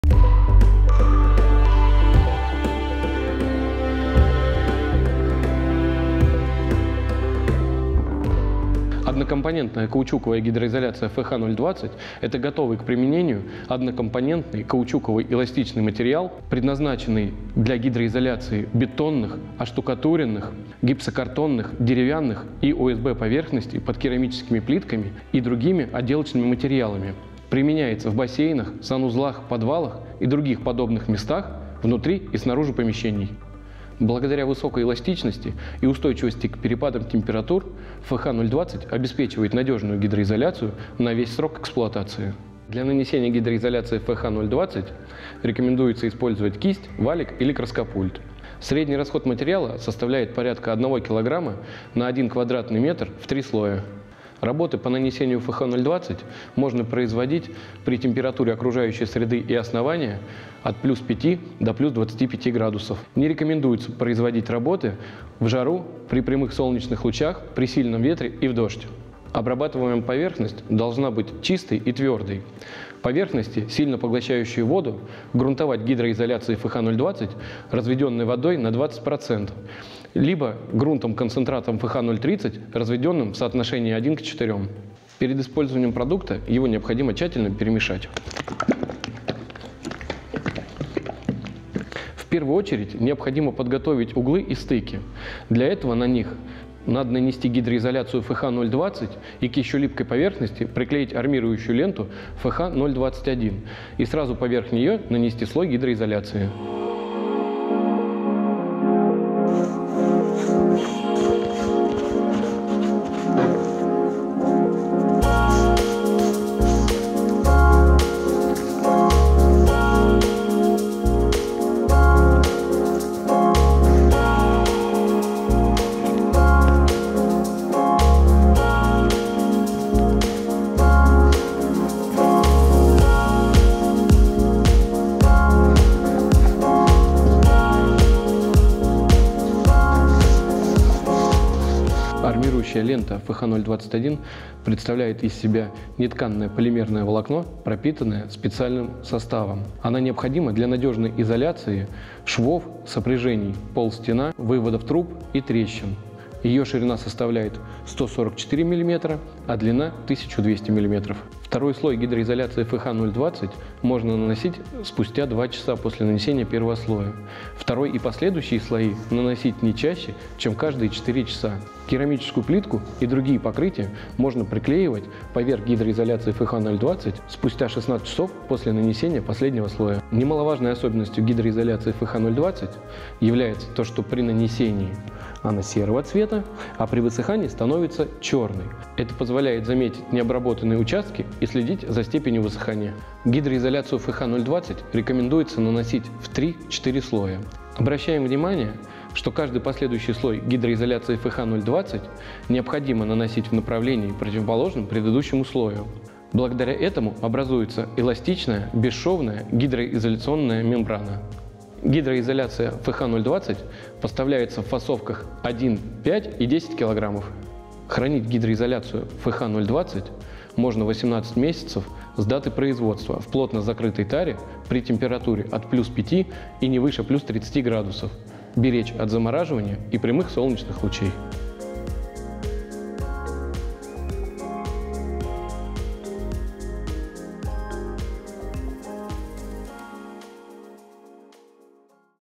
Каучуковая однокомпонентная гидроизоляция ФХ-020 и гидроизоляционная армирующая лента ФХ-021
Задавайте вопросы о продукции в комментариях!
Каучуковая однокомпонентная гидроизоляция ФХ-020 - готовый к применению состав на основе каучука. Обеспечивает отличную адгезию к материалам (древесина, бетон, камень, штукатурка, ГКЛ, ОСБ). Применяется для выполнения бесшовной гидроизоляции при наружных и внутренних работах. Используется совместно с гидроизоляционной армирующей лентой ФХ-021.

Ключевые преимущества:
1. ВЫСОКАЯ ЭЛАСТИЧНОСТЬ
Надежная преграда для воды даже при деформации основания.
2. МОРОЗОСТОЙКОСТЬ
Сохраняет все свойства после многократного замерзания.
3. ПАРОПРОНИЦАЕМОСТЬ
Принцип мембраны. Удерживает воду и пропускает пар.
4. УНИВЕРСАЛЬНОСТЬ
Бесшовная гидроизоляция при комбинировании разных материалов в одном помещении.
5. УДОБСТВО ПРИМЕНЕНИЯ
Малые интервалы между нанесением слоев позволяют сделать гидроизоляцию за сутки. Состав при полимеризации меняет цвет: исключены ошибки при нанесении последующих слоев.
6. ВЫСОКАЯ АДГЕЗИЯ
Применяется со всеми материалами кроме полиэтилена.
7. ГОТОВНОСТЬ К МОНТАЖУ ПЛИТКИ
Позволяет клеить плитку прямо на гидроизоляцию без грунтования.

Область применения: для внутренних и наружных работ

Тип поверхностей: санузлы, подвалы, бассейны, фонтаны, балконы

Расход: 0,2 – 0,5 кг/м2 в один слой

Состав: акрило-каучуковая дисперсия, силиконовая дисперсия, реологические добавки, микромрамор, консервант, вода

Гидроизоляционная армирующая лента ФХ-021 - применяемая совместно с каучуковой однокомпонентной гидроизоляцией ФХ-020, лента имеет оптимальные размеры. Малая толщина ленты позволяет усилить углы и примыкания без утолщений и складок. Удобна в работе за счет отсутствия жесткой середины.

Ключевые преимущества:
1. ПАРОПРОНИЦАЕМОСТЬ
Работает по принципу мембраны: пропуская пар, препятствует разрушению материалов.
ОПТИМАЛЬНЫЕ РАЗМЕРЫ
Ширина, достаточная для формирования нахлестов. Длина, удобная для раскроя без остатка. Толщина менее 0,5 мм позволяет минимизировать кривизну стен.
УДОБСТВО ПРИМЕНЕНИЯ
Тонкая, гибкая, эластичная лента не требует применения специального инструмента.

Тип поверхностей: примыкания стен и полов, углы, выводы труб